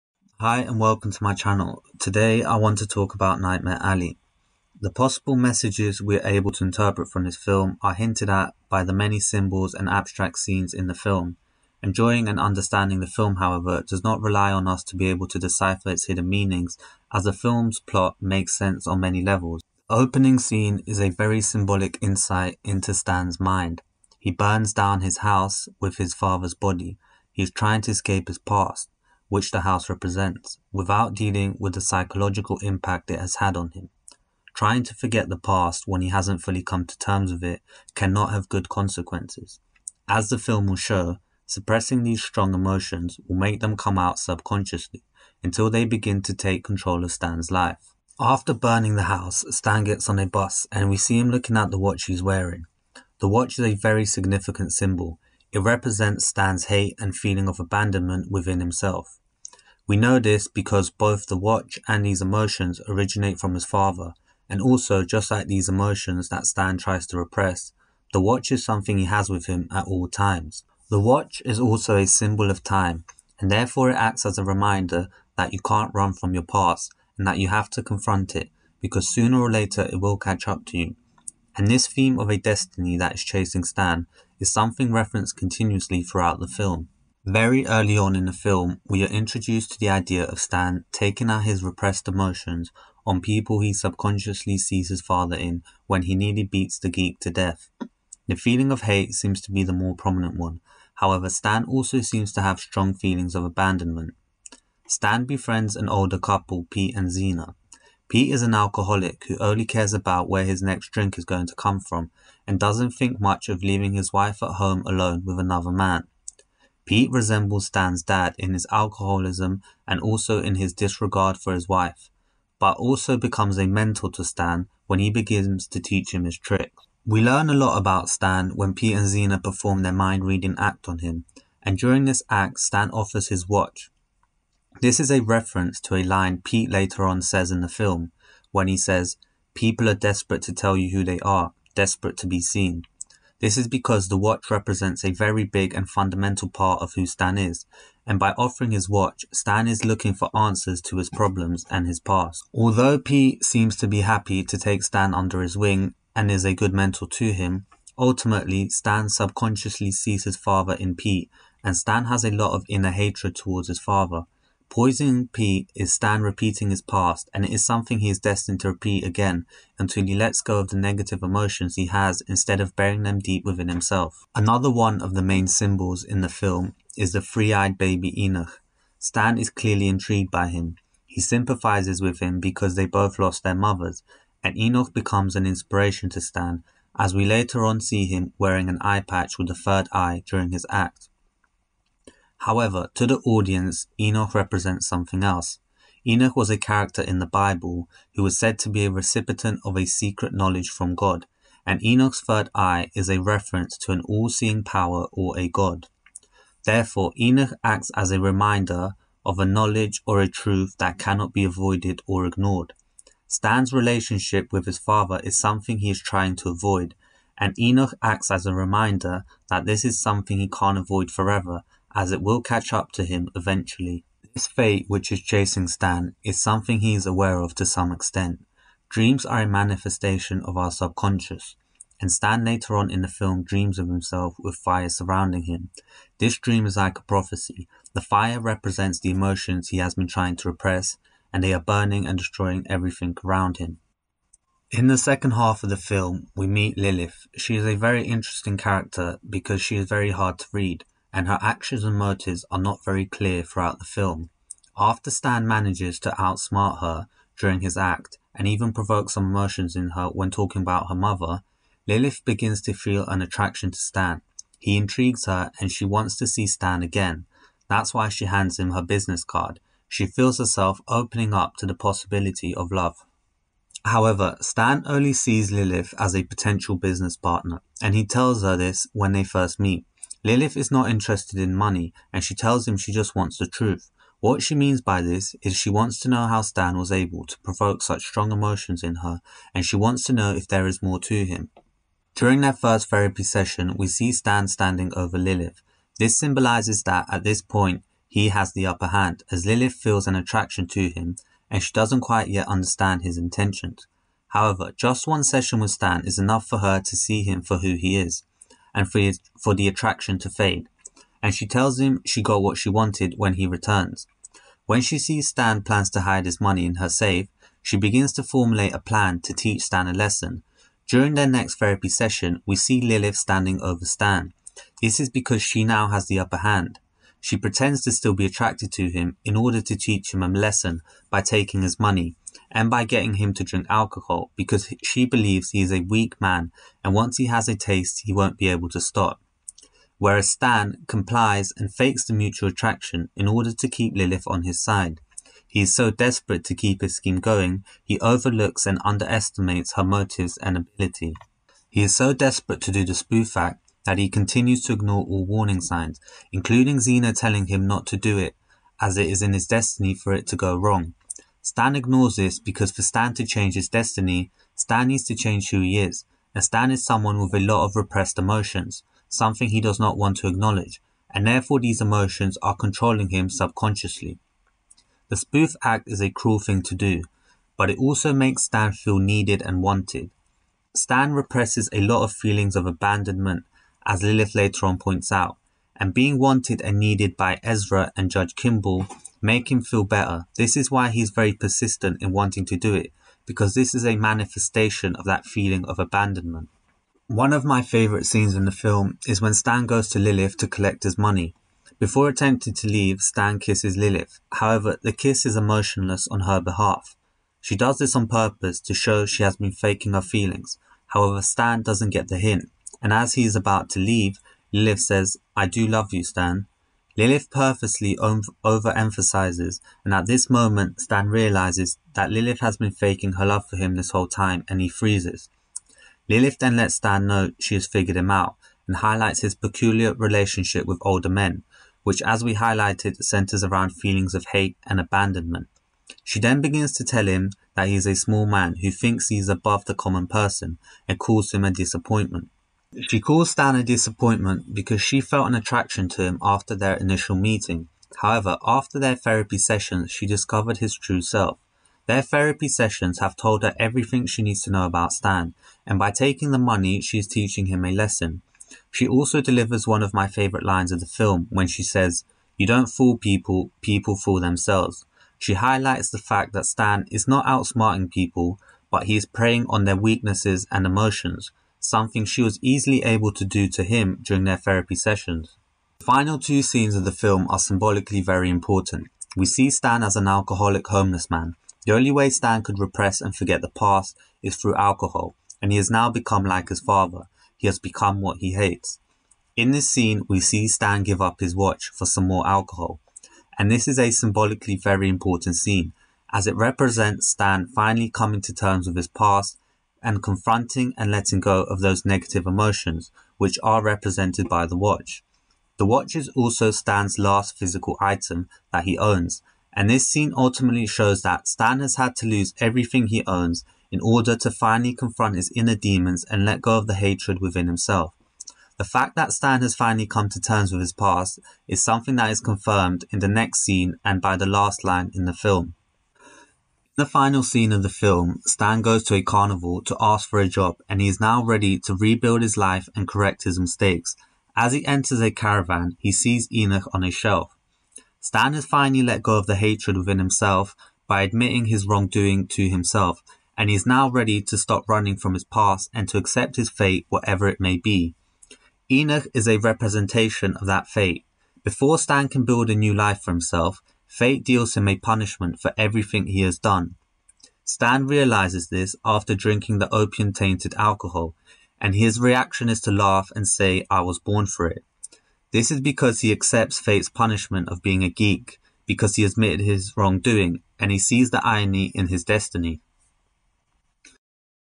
Nightmare Alley - Symbols, Meanings and Ending Explained - Analysis Review -
In this video I take a look at the meanings and symbols in the film Nightmare Alley, let me know what you thought of my interpretations and if there is anything I missed out.